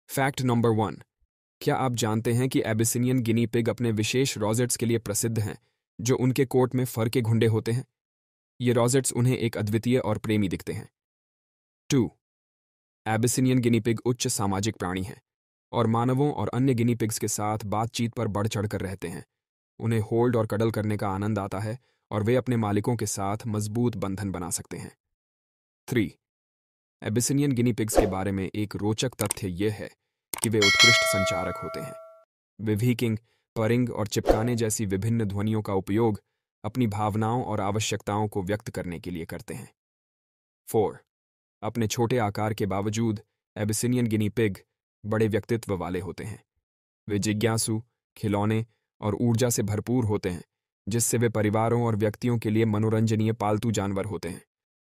Discovering Fascinating Facts about Abyssinian Guinea Pig.
"Embark on an enlightening exploration of the Abyssinian Guinea Pig with our YouTube short video, "Discovering Fascinating Facts about Abyssinian Guinea Pig!" 🐹🌿 Dive into the intriguing world of these adorable rodents, from their distinctive coat patterns and playful personalities to their history as beloved pets and care tips for prospective owners. Whether you're a guinea pig enthusiast or simply curious about small pets, this video provides a delightful glimpse into the unique characteristics of Abyssinian Guinea Pigs.

Join us on this educational journey as we uncover the captivating facts that make Abyssinian Guinea Pigs such charming companions! 🌟🐹"